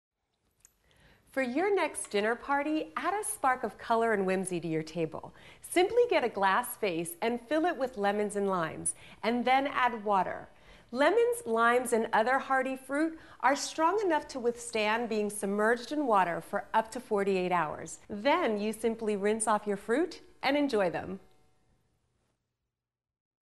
Home Decor Ideas : Lemon Lime Centerpiece Idea - Home Made Simple
Get more Home Decor Ideas from Home Made Simple To add a sparkle of color and whimsy to your outdoor living space, fill a small fishbowl with kumquats and limes up to the middle of the pot. Then, add water and any flower clippings you have around your yard to fill the fishbowl. Both kumquats and limes are small enough to fit into the narrow fish bowl's opening and hearty enough to withstand being submerged in water. Its a fun centerpiece that uses nothing but Mother Nature's gifts, and brings a little more sunshine into your space.